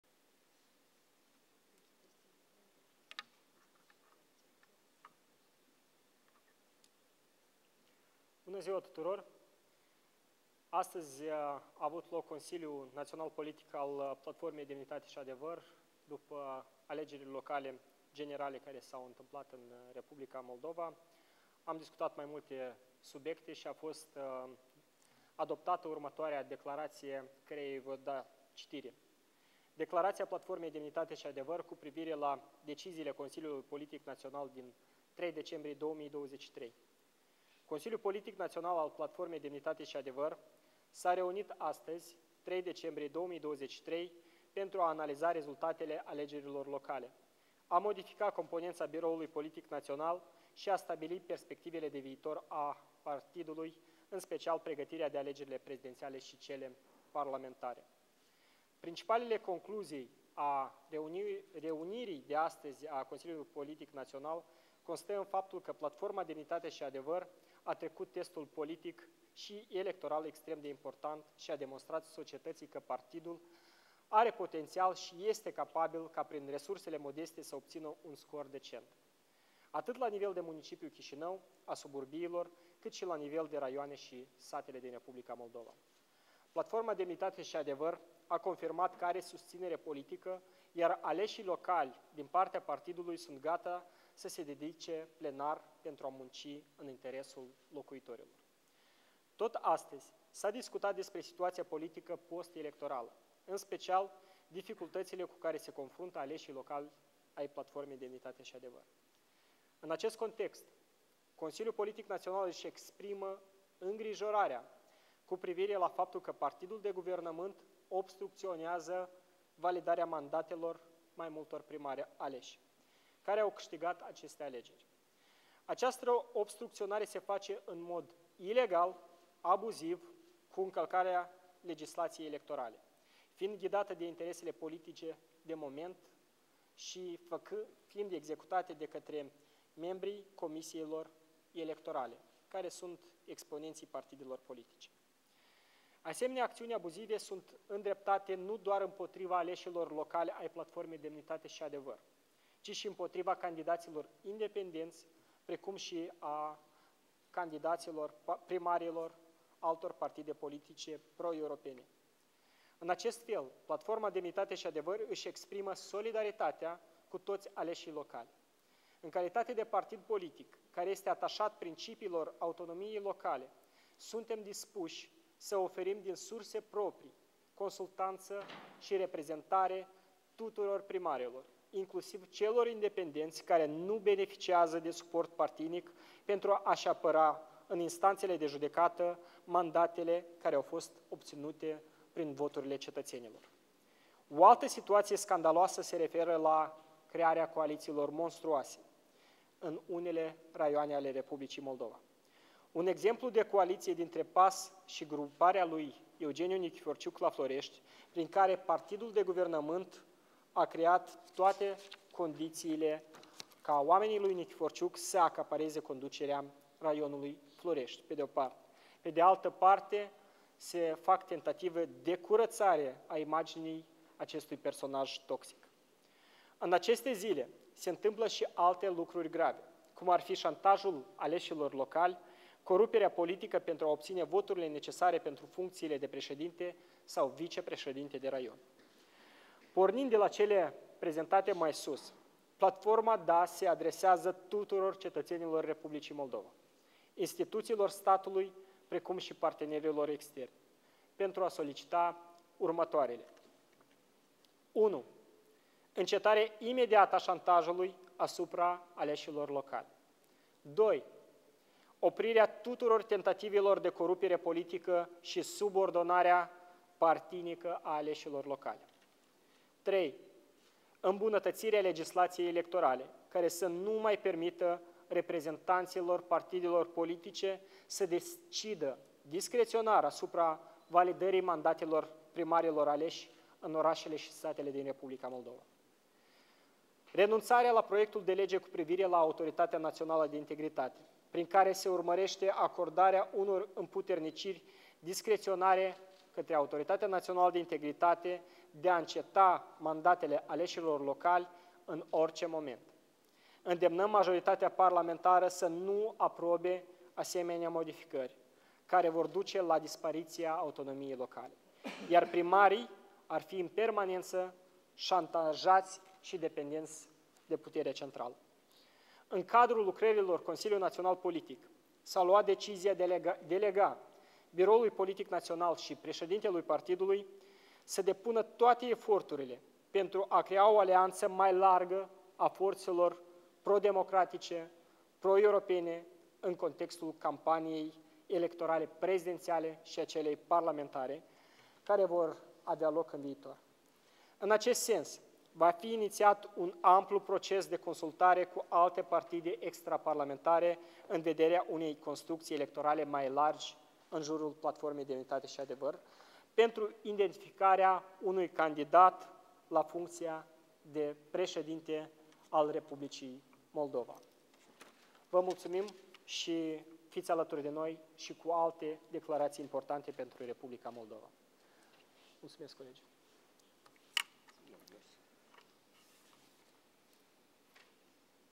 Conferință de presă organizată de Partidul Platforma Demnitate și Adevăr după ședința CNP
Conferință de presă organizată de Partidul Platforma Demnitate și Adevăr după ședința Consiliului Național Politic - 3 decembrie 2023